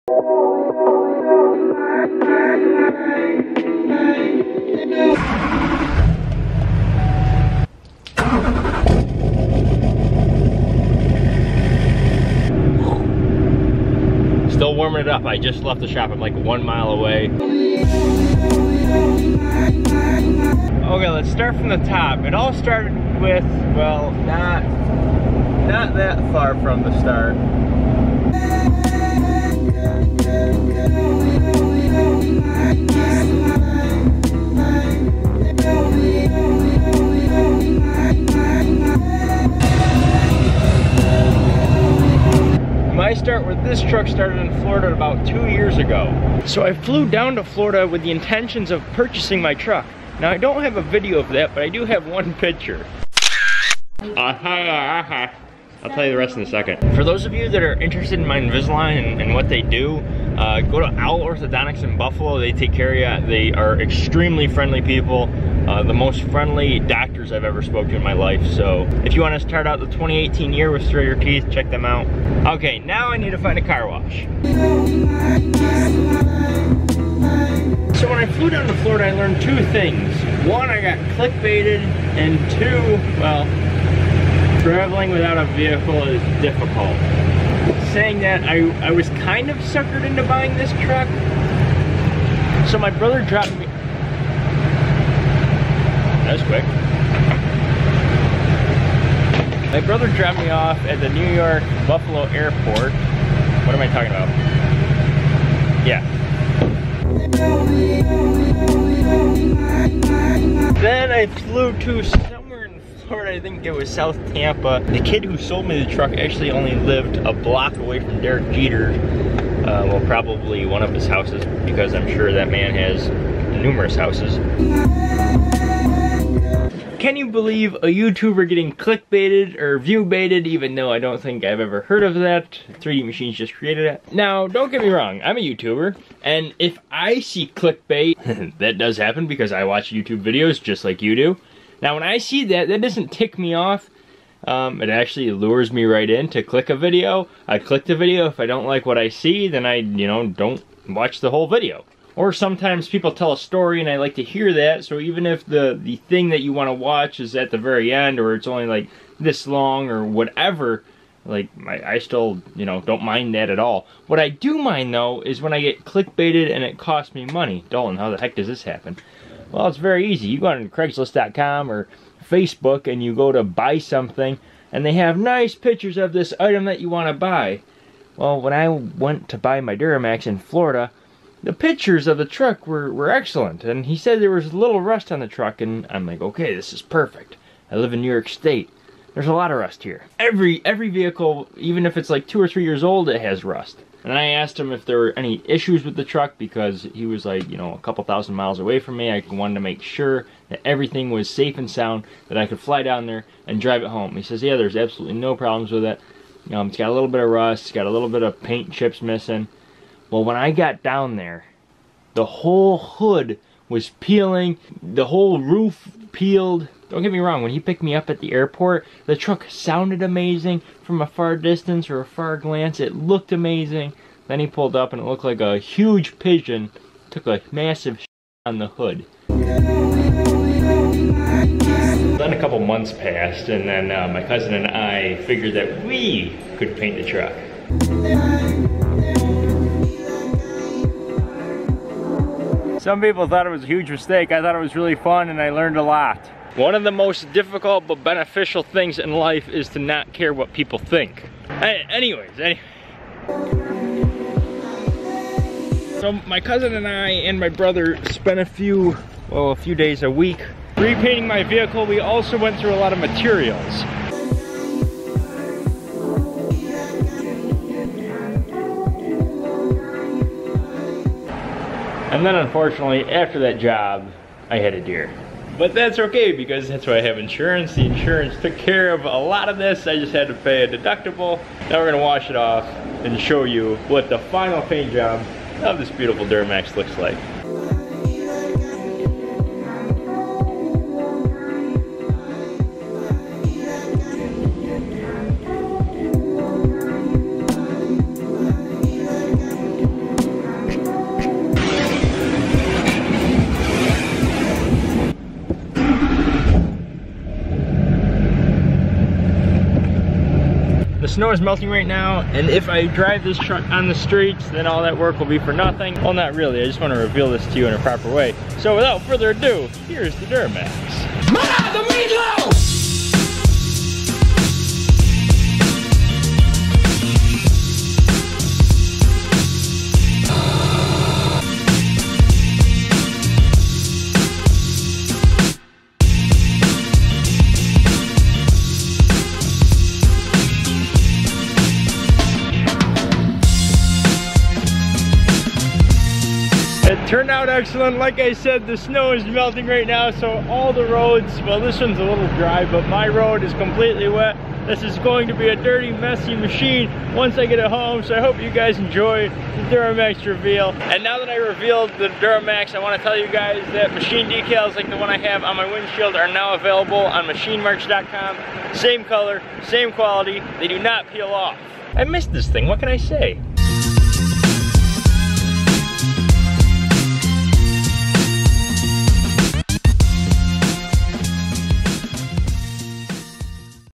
My NEW DURAMAX Reveal!
It's been a long wait but here's my new duramax... again...
This is what my new Duramax looks like. I have a bumper on order. I look forward to installing that soon.

FAN MAIL:
3DMachines